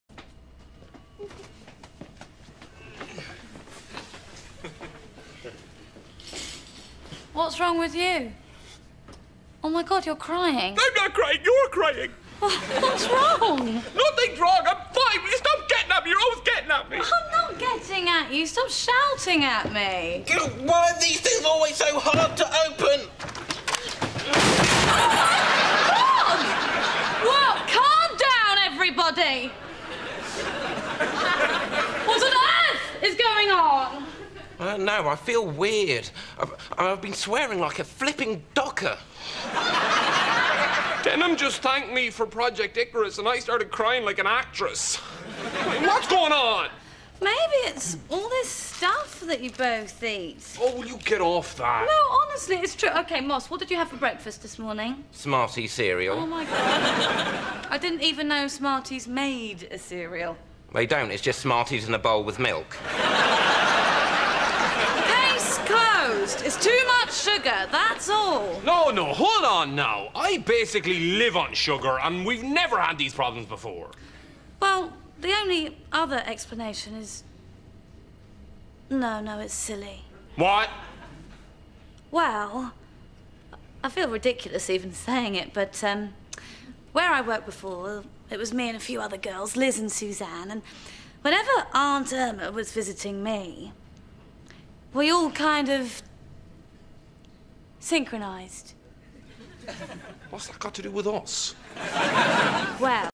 Smarties Cereal
I love the smell of smarties in a bowl with milk in the morning. It smells like...sugar rush.